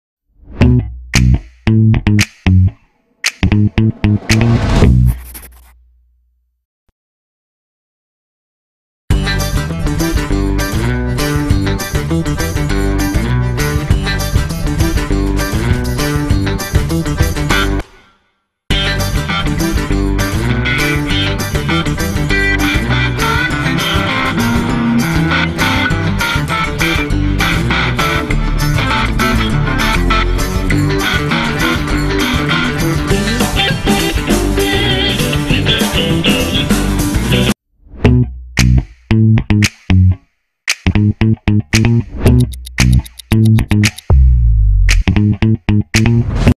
Mickey Mouse - Red Cheek Mega Jumbo Lying Down Pose Plushy #1
Second Mickey Mouse - Red Cheek Mega Jumbo Lying Down Pose Plushy (ミッキーマウス　赤いほっぺ　メガジャンボ寝そべりポーズふわろんぬいぐるみ) won this week!